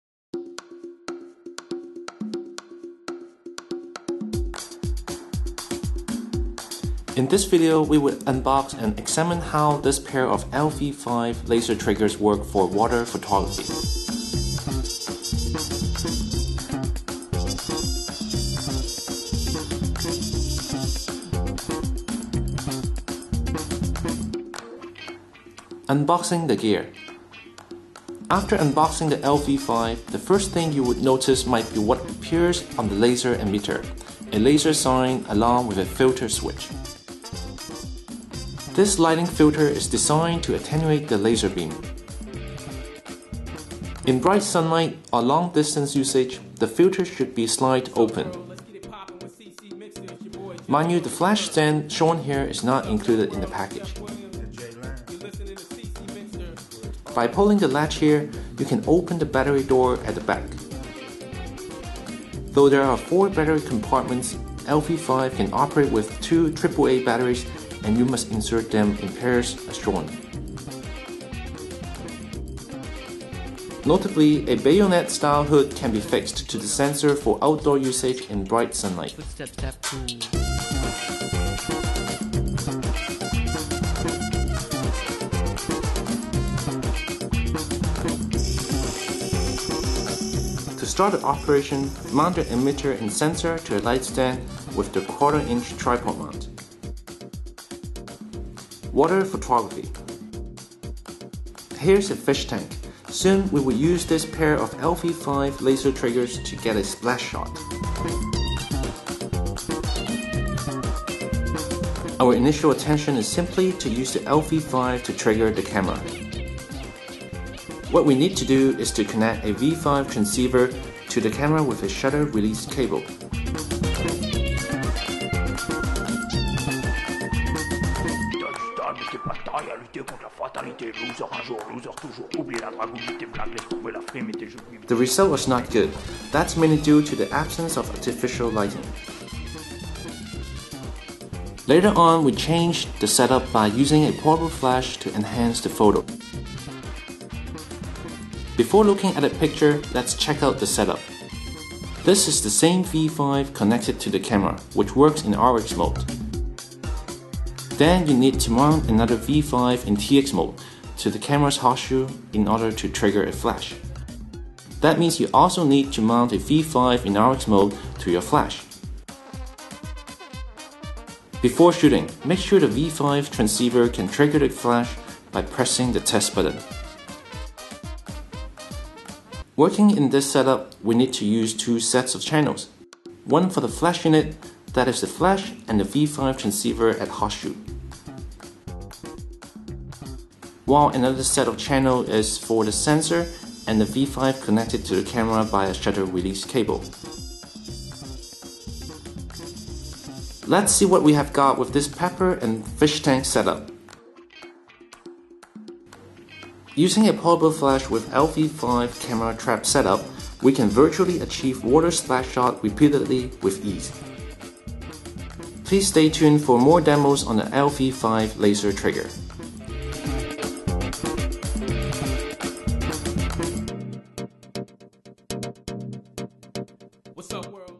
Product demo: How to use Cactus LV5 laser triggers for water photography with portable flash?
We demonstrate the skills to achieve water splash shots with LV5 and V5 transceivers along with our portable flash with ease.